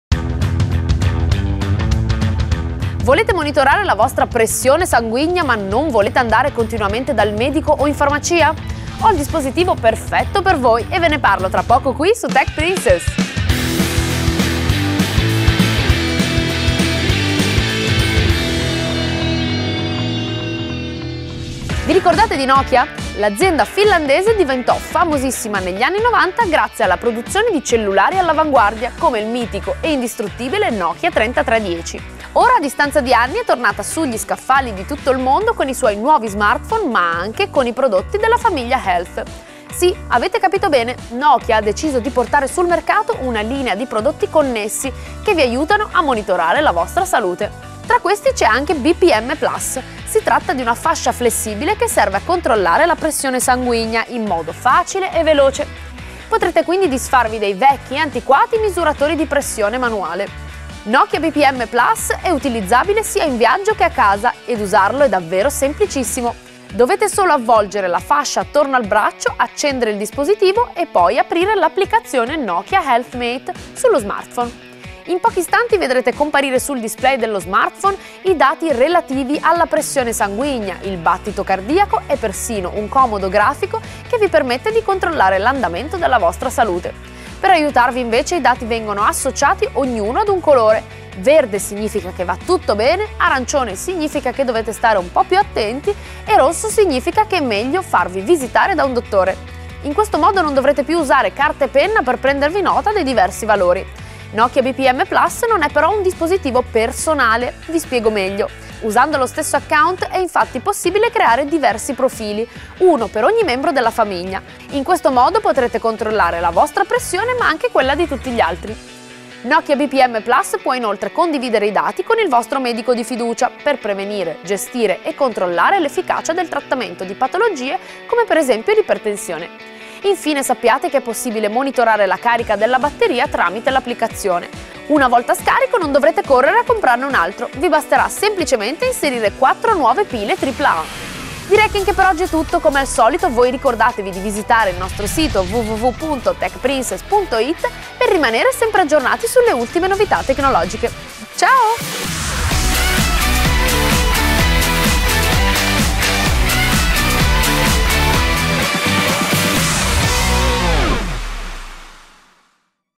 Nokia BPM+: il misuratore di pressione smart 📺
Tech Princess sbarca finalmente in TV! 📺
In questa puntata la nostra Fjona Cakalli vi parlerà di Nokia BPM+, un misuratore di pressione smart. Vi spieghiamo come funziona.